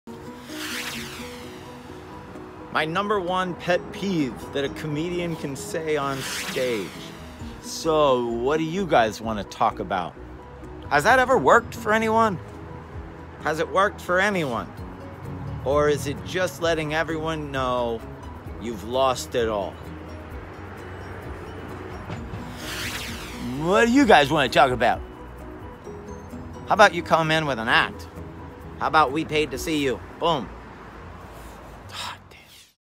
Dante Comedy Tip #16 Never ask the audience what they want to talk about
Dante has been a comedian since 1986.  He has appeared on over 40 tv shows. Including being a regular on BETs Comic View and a cast member on NBCs Last Comic Standing. He is now also a celebrity entertainment manager in Hollywood.
DANTE COMEDY TIP #16

Never ask the audience what they want to talk about.